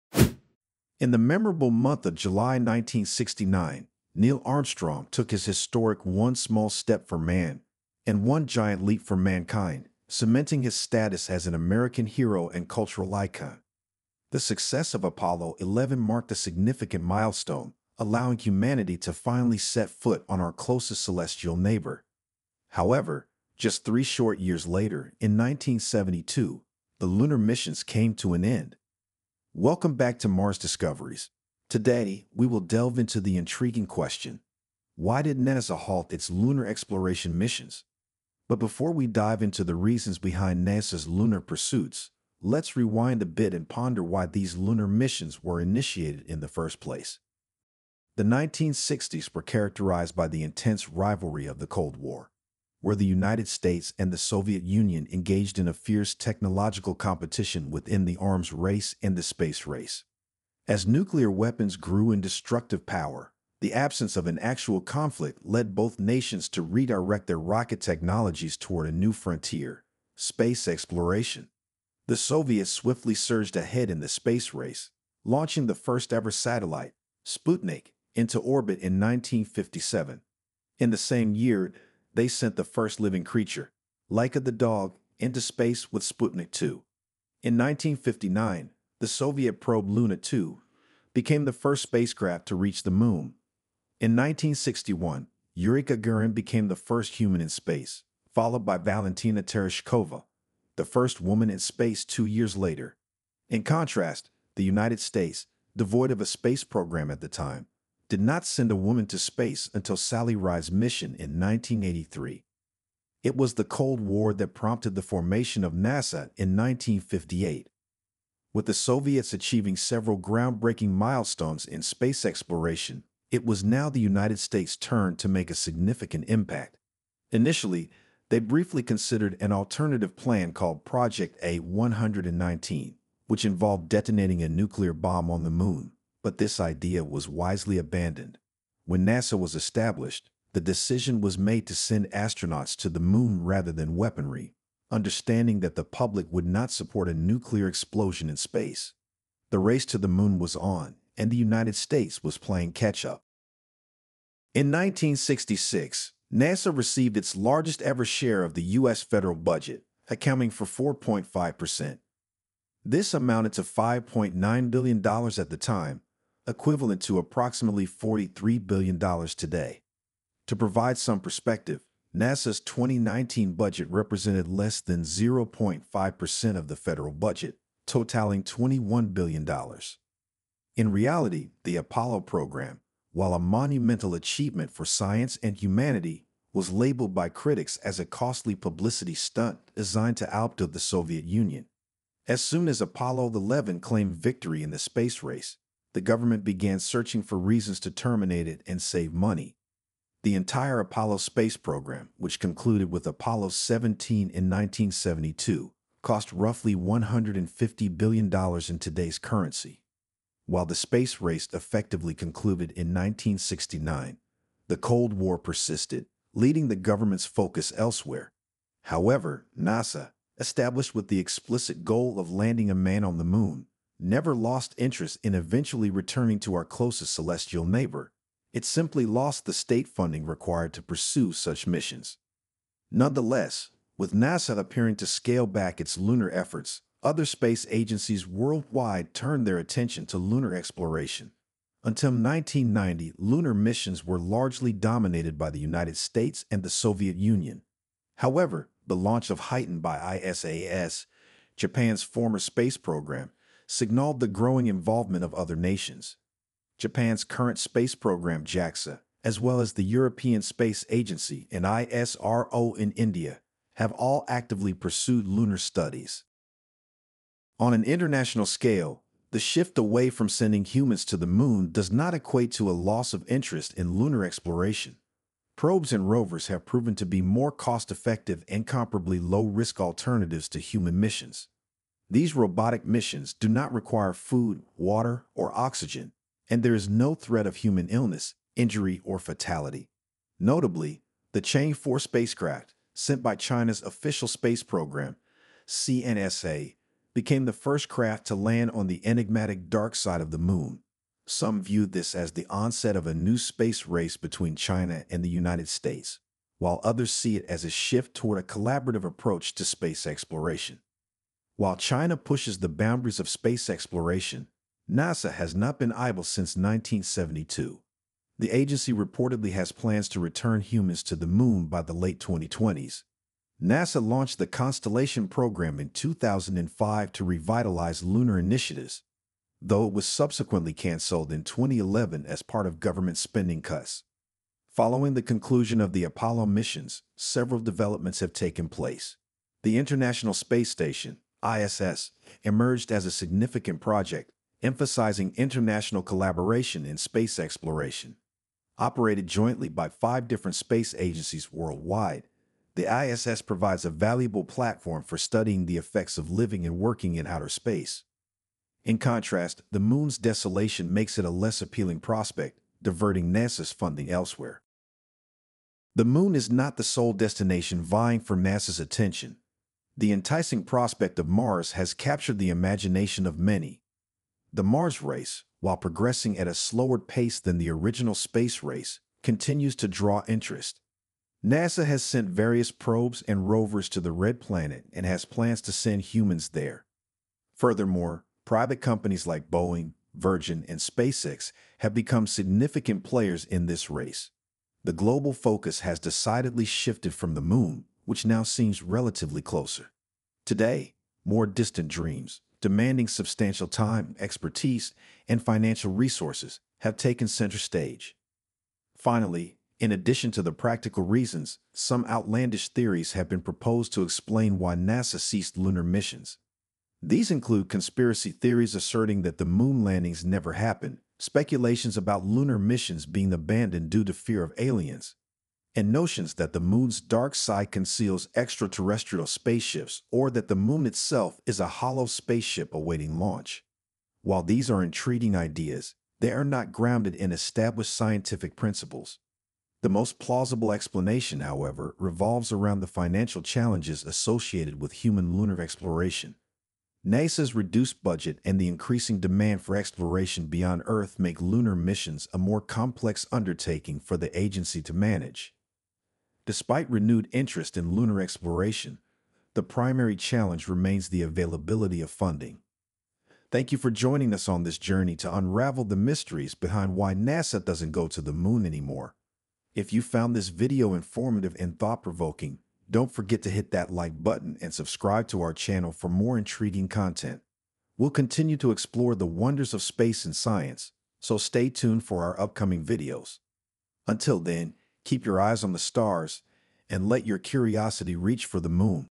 The Untold Story: NASA's Decision to Pause Moon Missions
Welcome to a fascinating journey into the untold story behind NASA's decision to pause moon missions!  In this video, we dive deep into the archives to uncover the hidden reasons and intricate details that led to the interruption of lunar exploration.

 Join us as we open the pages of history and explore the lesser-known factors that contributed to NASA's surprising decision. From technical challenges to shifting priorities, we unveil the complexities that shaped this pivotal moment in space exploration.